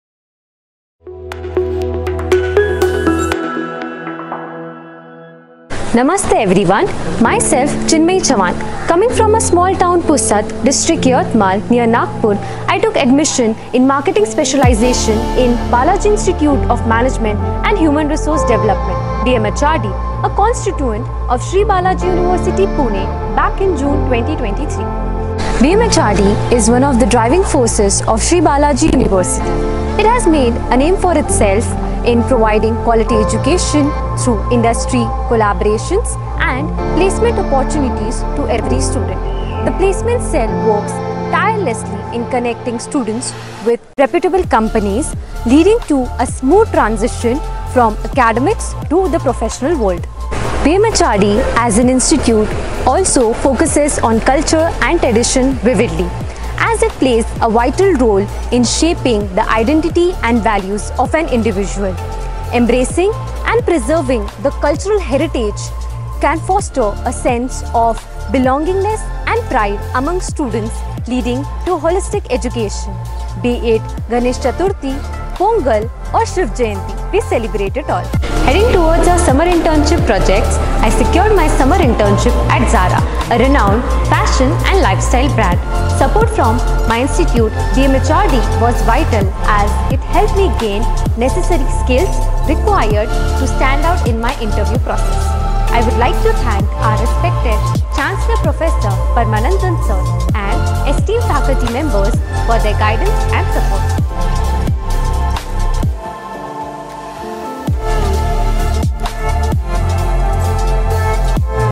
A New Leaf Of BIMHRD | Student manager of BIMHRD 2
Journey of Campus to corporate, Student manager of BIMHRD

Video credits : Shivam karodiya
prateek chechani